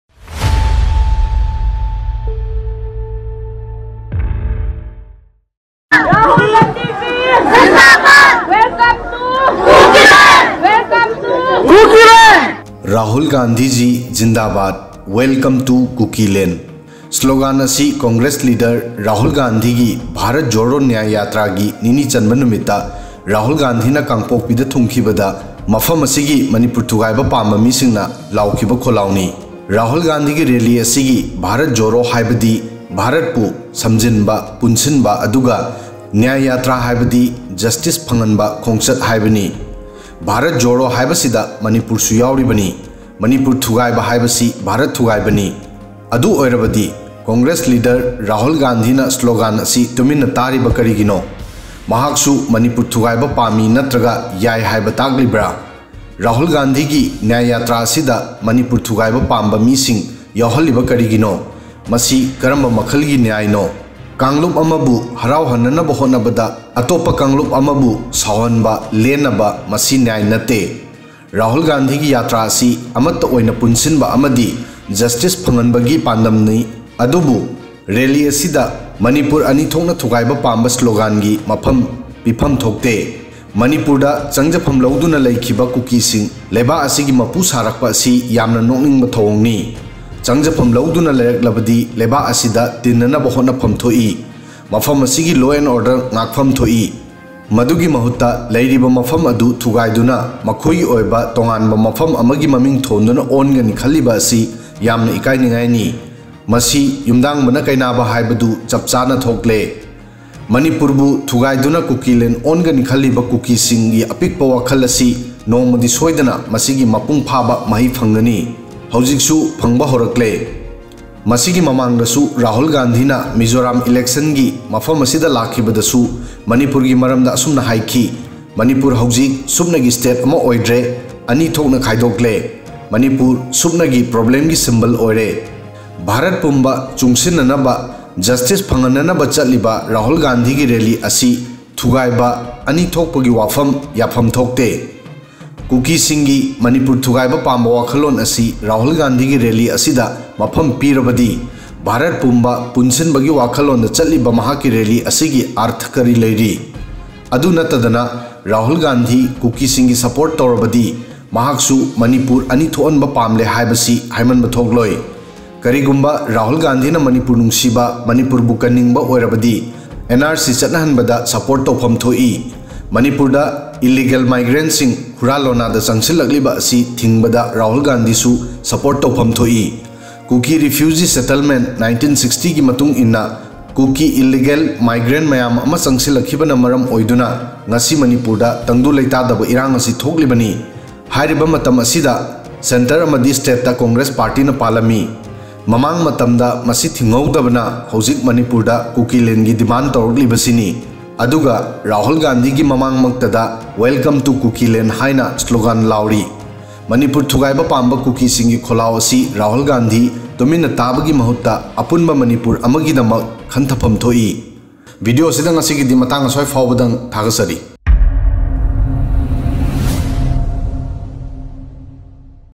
Rahul Gandhi gi Nyay Yatrada Manipur Thugaiba Pamba Meesing I Connect News
"Rahul Gandhi Ji Jindabad, Welcome to Kukiland" Slogan asi Congress Leader Rahul Gandhi gi Bharat Jodo Nyan Yatra gi gi nini chanba numita Rahul Gandhi na  Kangpokpida thungkhibada mafam asigi Manipur thugaiba pamba meesingna laokhiba kholaoni.